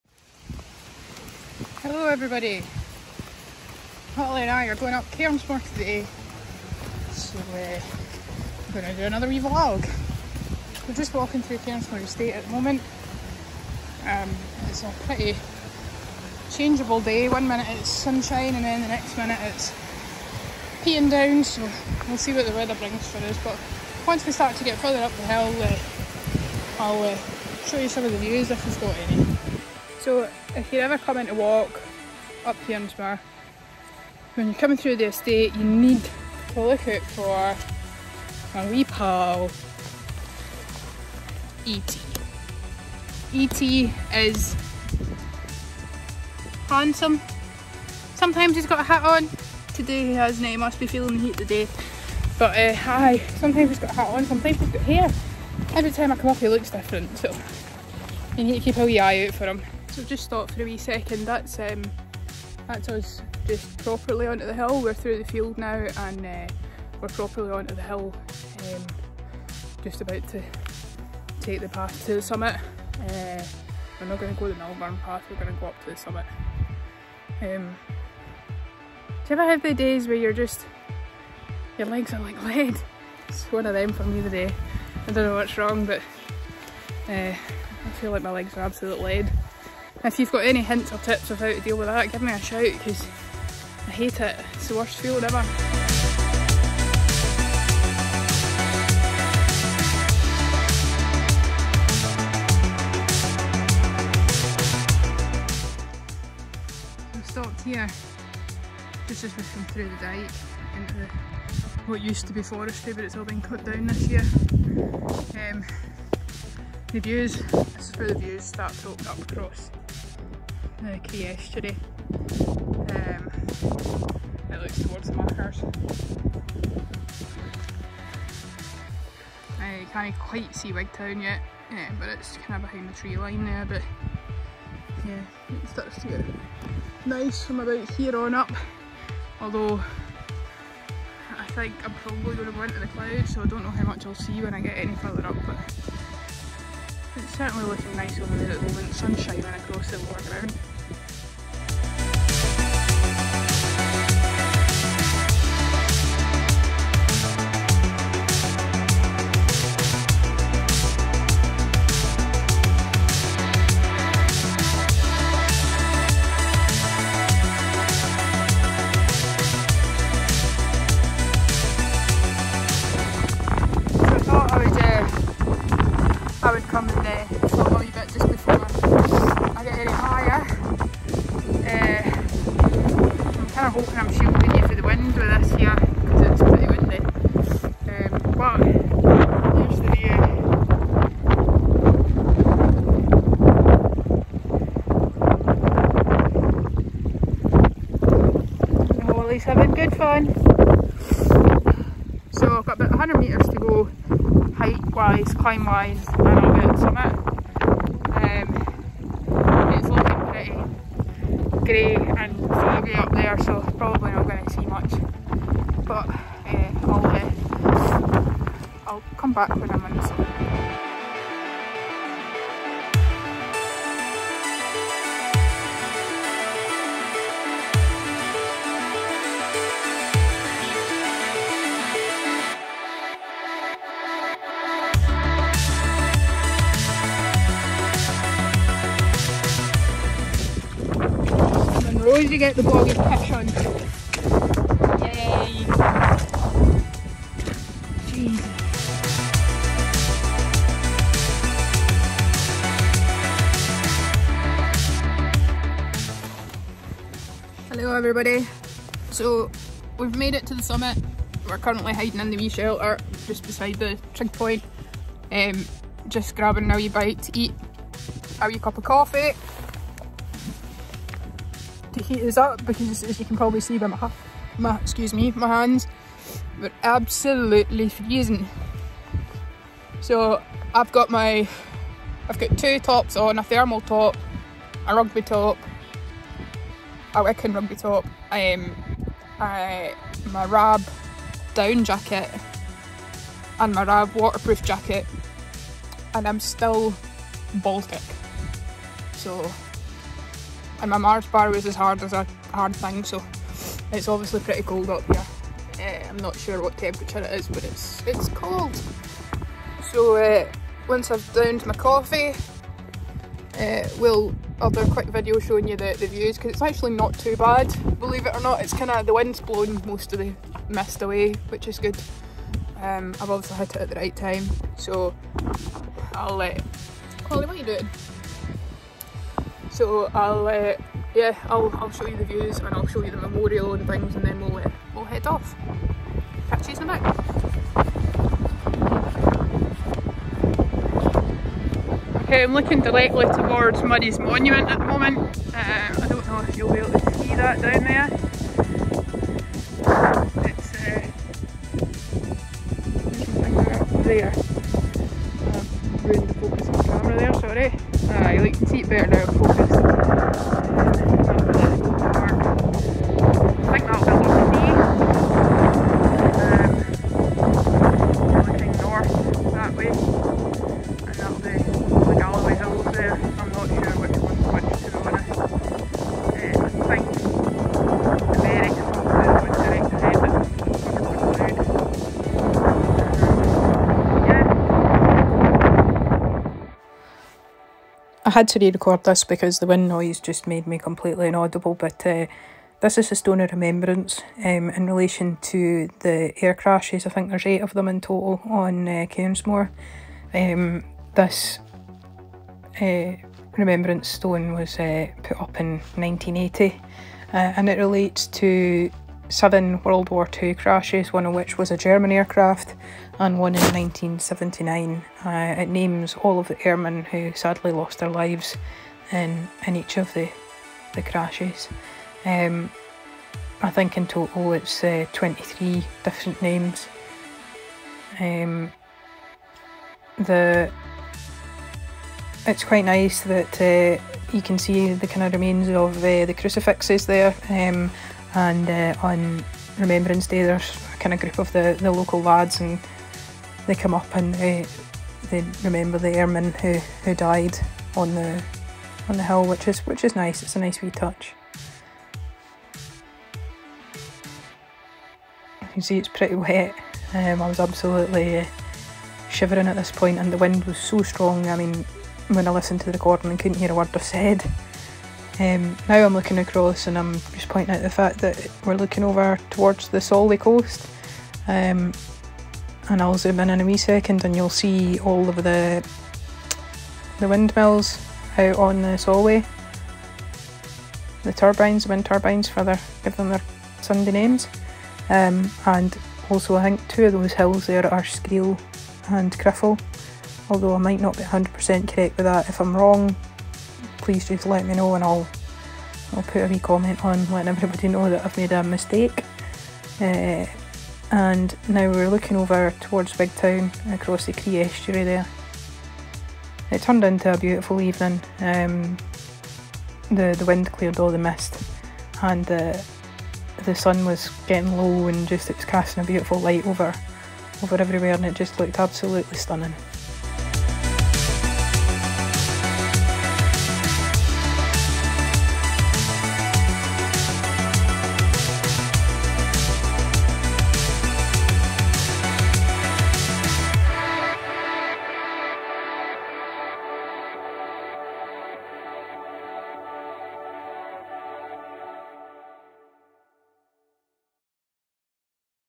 Hiking In Scotland: Cairnsmore of Fleet
Join Holly and I as we hike to the summit of Cairnsmore of Fleet on a rather blustery autumn day!

Cairnsmore of Fleet is classed as a Donald with a height of 711m above sea level. It affords great views over the Cree Estuary and the Machars and to the North; the Galloway Hills, including the Merrick as well as Loch Dee.